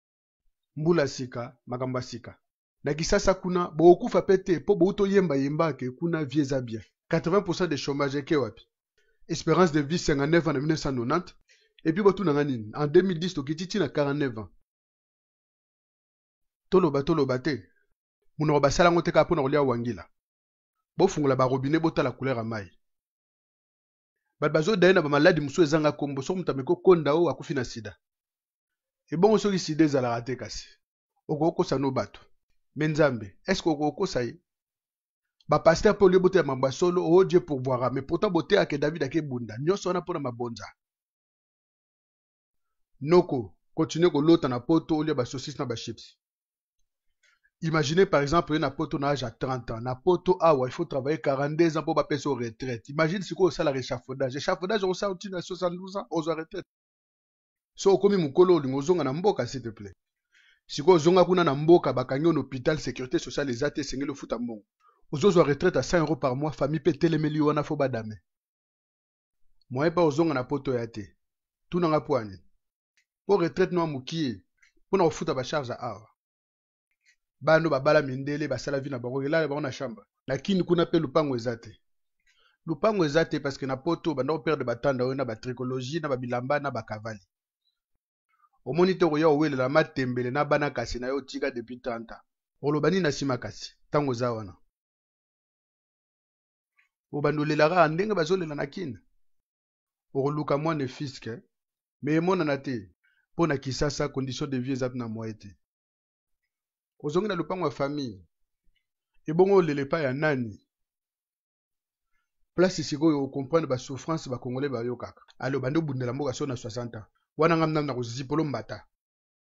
FLASH: MESSAGE DE OBAMA ADRESSÉ AUX DIASPORA CONGOLAIS & AUX KINOIS AINSI QU'ÀU PRESIDENT FATSHI
FLASH: MESSAGE DE BARACK OBAMA ADRESSÉ AUX DIASPORA CONGOLAIS & AUX KINOIS AINSI QU'ÀU PRESIDENT FATSHI